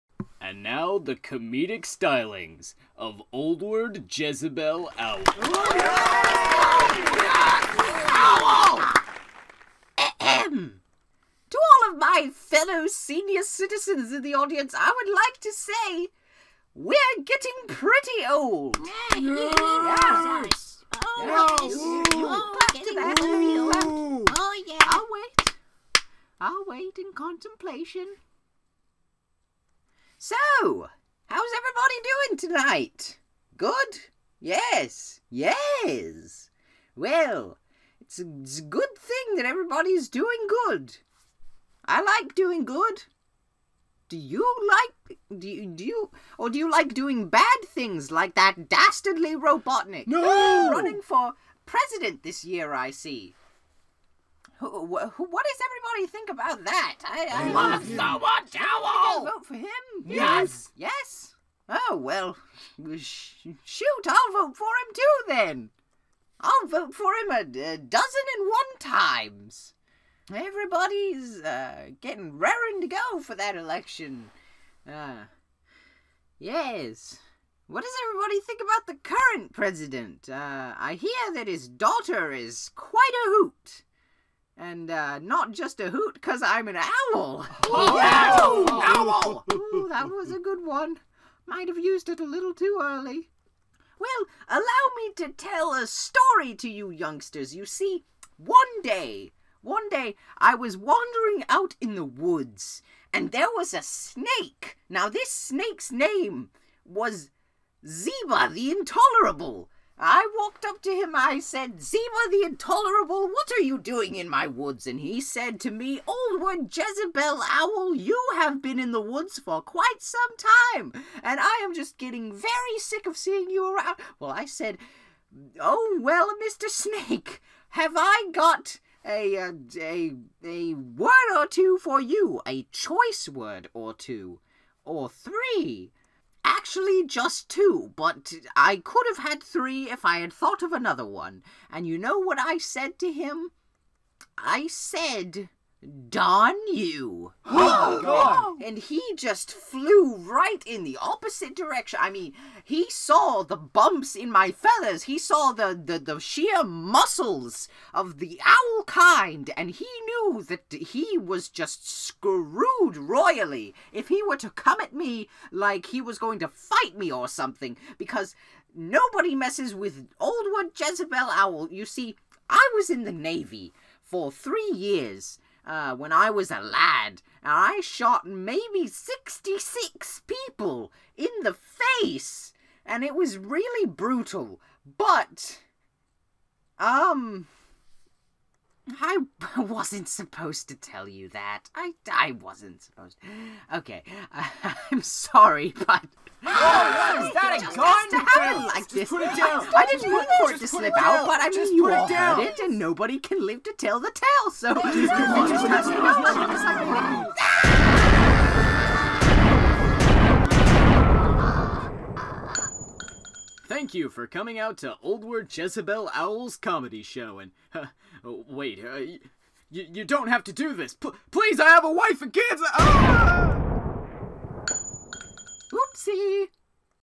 The Comedic Stylings of Old Man Owl
A sample of Oldward Jezebel Owl's world famous standup comedy.

Created and performed by SilentlyVocal